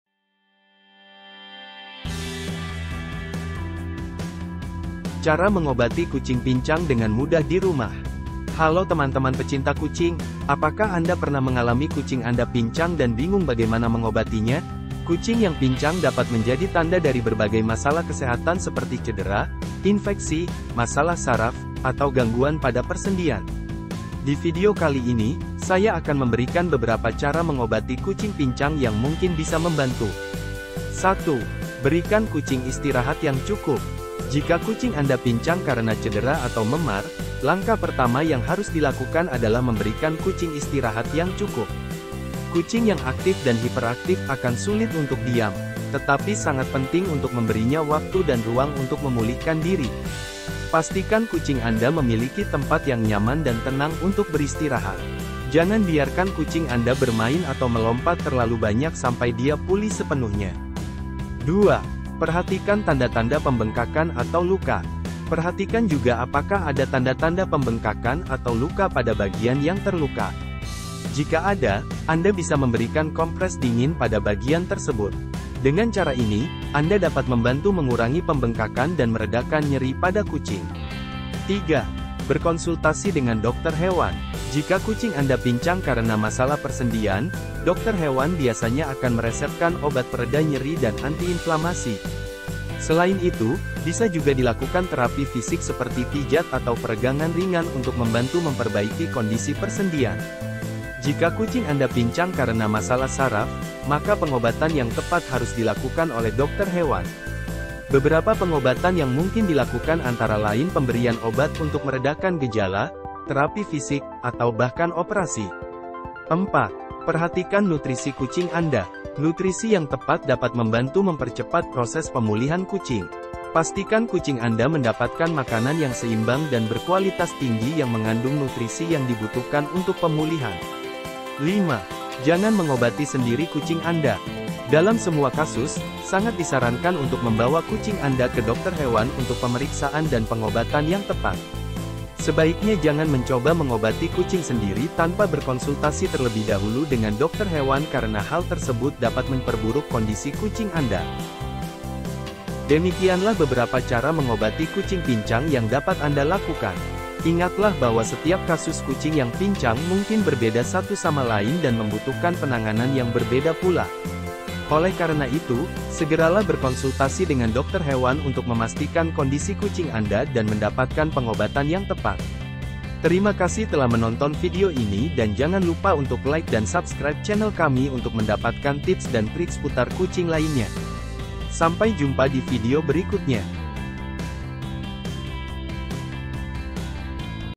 Cara Mengobati Kucing Pincang dengan Mudah di Rumah
Apakah kucing Anda pincang dan Anda bingung bagaimana mengobatinya? Jangan khawatir, di video ini kami akan memberikan beberapa cara mengobati kucing pincang yang bisa dilakukan di rumah. Mulai dari memberikan istirahat yang cukup, perawatan cedera, pemberian nutrisi yang tepat, hingga berkonsultasi dengan dokter hewan. Selain itu, kami juga akan membahas berbagai masalah kesehatan yang dapat menyebabkan kucing pincang seperti masalah persendian, masalah saraf, atau infeksi. Video ini akan memberikan tips dan trik yang efektif untuk membantu kucing Anda sembuh dari pincang dan kembali beraktivitas dengan normal.